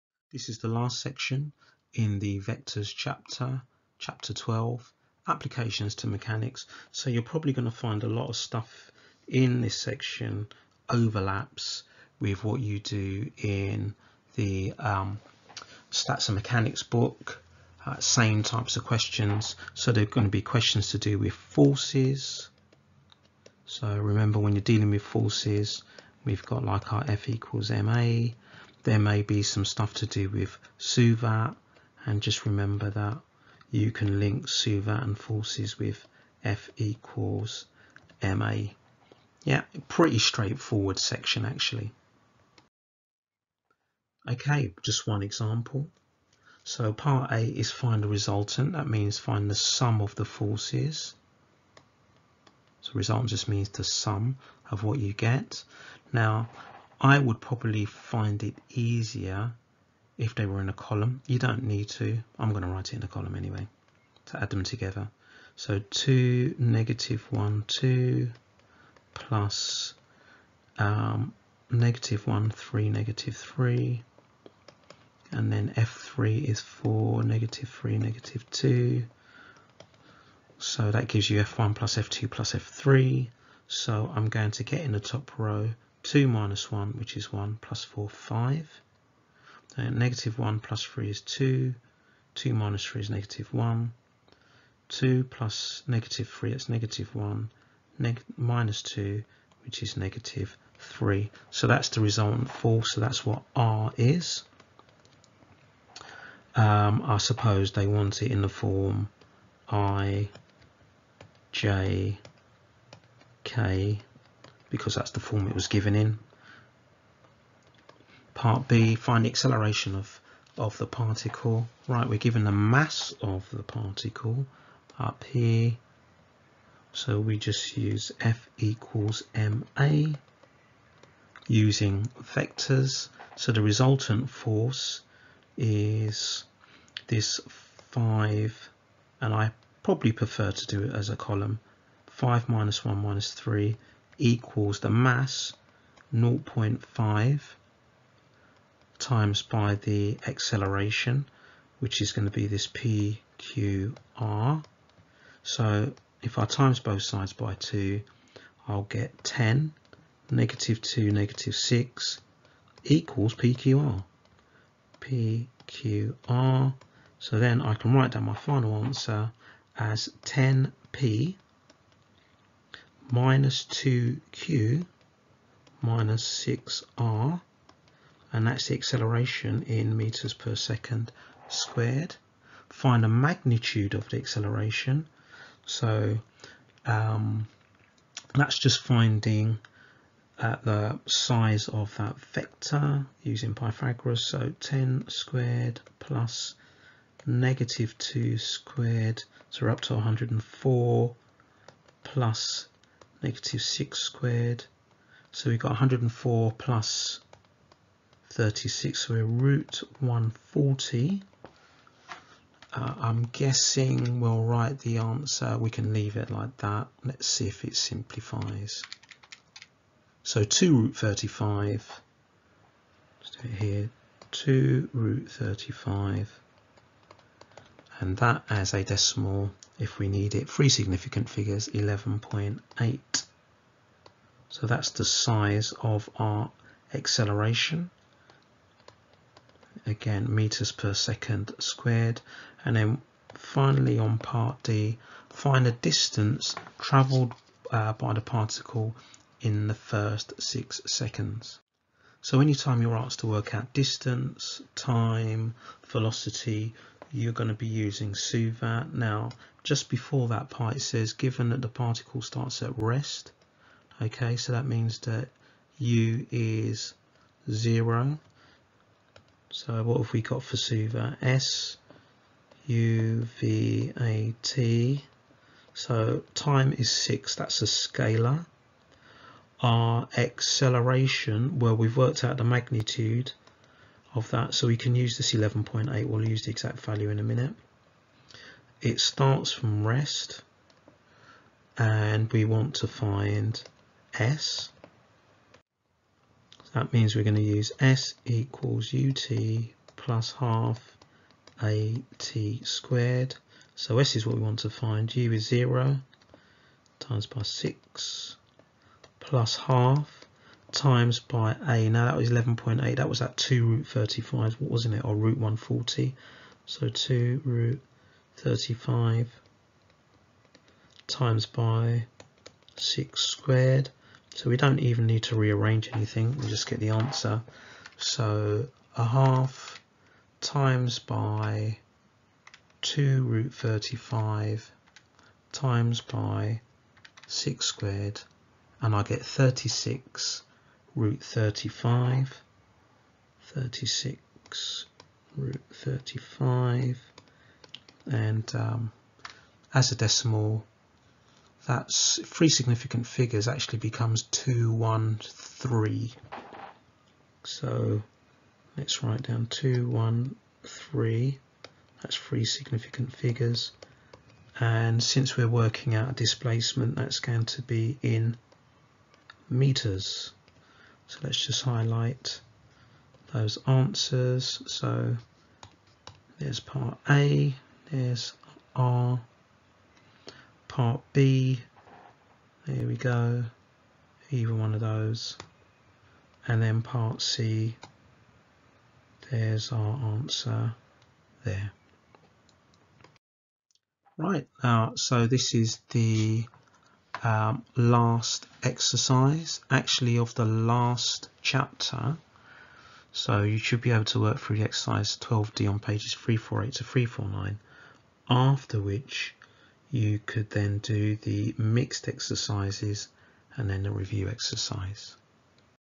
12.4 Applications to Mechanics (PURE 2 - Chapter 12: Vectors)
hindsmaths Applying 3 dimensional vectors to mechanics
0:00 Intro
0:42 Example 13
6:33 End/Recap